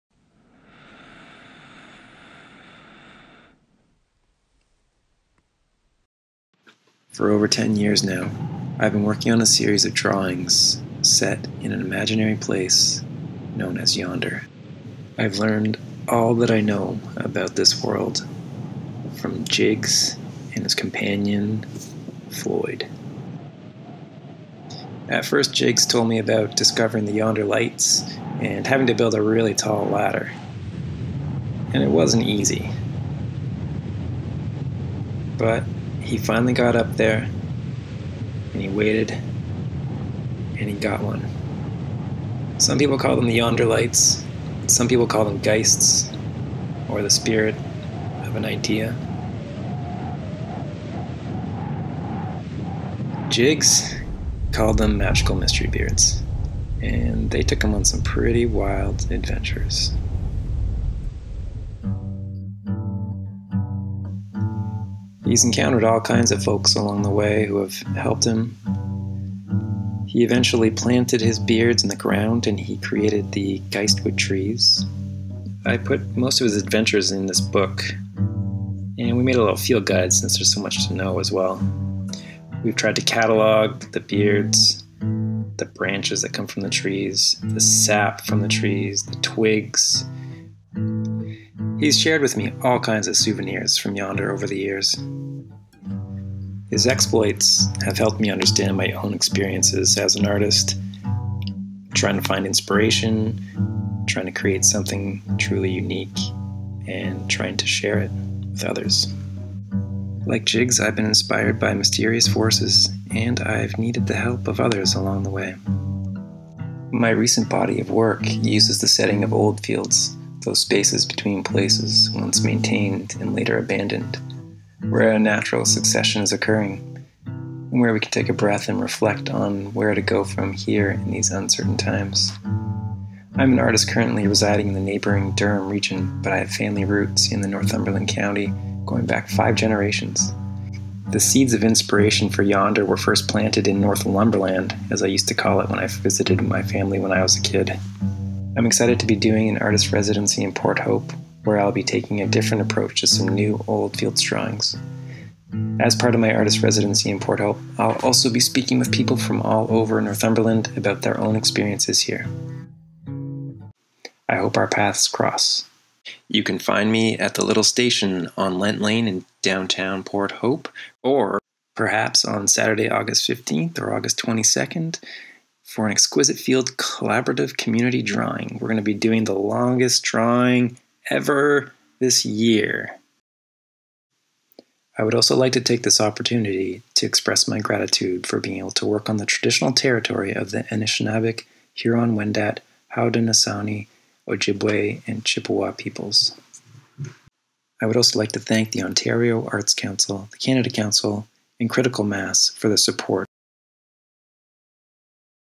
Hope Project promo FINAL HQ
Critical Mass is excited to introduce you to our first artist-in-residence — Jay Dart — who will be doing his residency at the Little Station, in Lent Lane Port Hope from August 10-22, 2020!